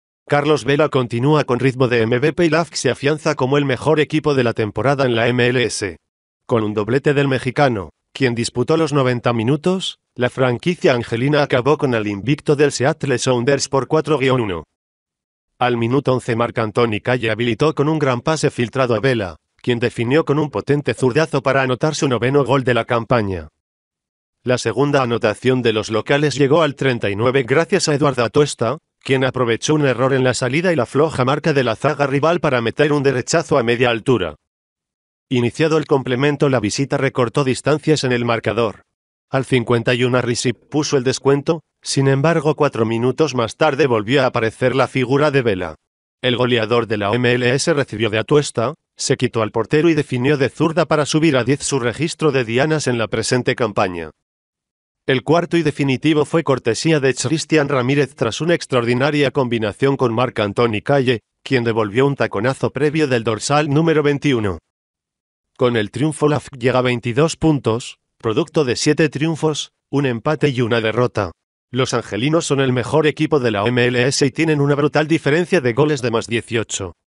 Con doblete de Carlos Vela el LAFC vence 4-1 al Seattle Sounders y les quita el invicto en la MLS
Carlos Vela continúa con ritmo de MVP y LAFC se afianza como el mejor equipo de la temporada en la MLS. Con un doblete del mexicano, quien disputó los 90 minutos, la franquicia angelina acabó con el invicto del Seattle Sounders por 4-1.

Al minuto 11 Mark-Anthony Kaye habilitó con un gran pase filtrado a Vela, quien definió con un potente zurdazo para anotar su noveno gol de la campaña.

La segunda anotación de los locales llegó al 39 gracias a Eduard Atuesta, quien aprovechó un error en la salida y la floja marca de la zaga rival para meter un derechazo a media altura.

Iniciado el complemento la visita recortó distancias en el marcador. Al 51 Harry Shipp puso el descuento; sin embargo cuatro minutos más tarde volvió a aparecer la figura de Vela. El goleador de la MLS recibió de Atuesta, se quitó al portero y definió de zurda para subir a 10 su registro de dianas en la presente campaña.

El cuarto y definitivo fue cortesía de Christian Ramírez tras una extraordinaria combinación con Mark-Anthony Kaye, quien devolvió un taconazo previo del dorsal número 21.

Con el triunfo LAFC llega a 22 puntos, producto de 7 triunfos, 1 empate y 1 derrota. Los angelinos son el mejor equipo de la MLS y tienen una brutal diferencia de goles de +18.